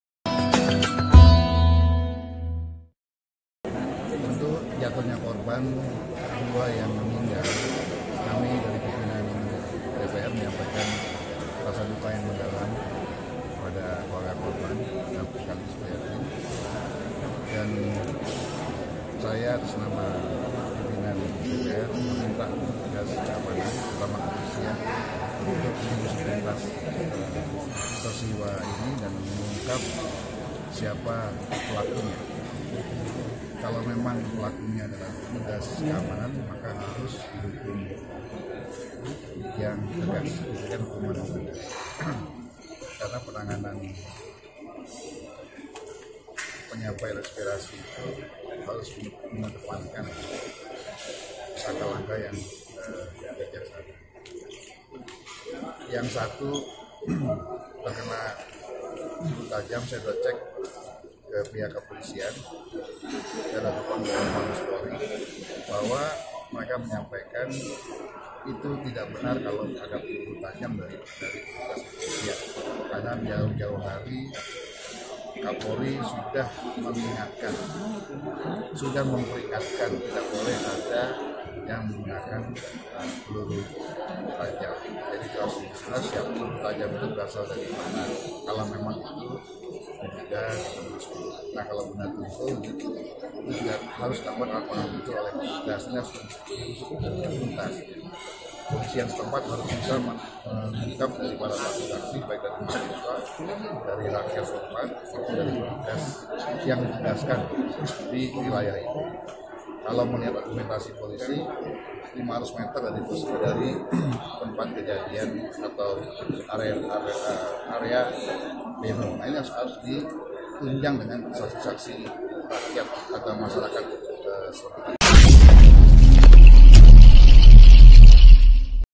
Polri Harus Usut Tuntas Tewasnya 2 Mahasiswa Universitas Halu Oleo
Ketua DPR RI, Bambang Soesatyo (Bamsoet) meminta Kapolri bertindak tegas dalam mengusut tuntas tewasnya  2 orang mahasiswa pada aksi demonstrasi tolak RKUHP di Sulawesi Tenggara.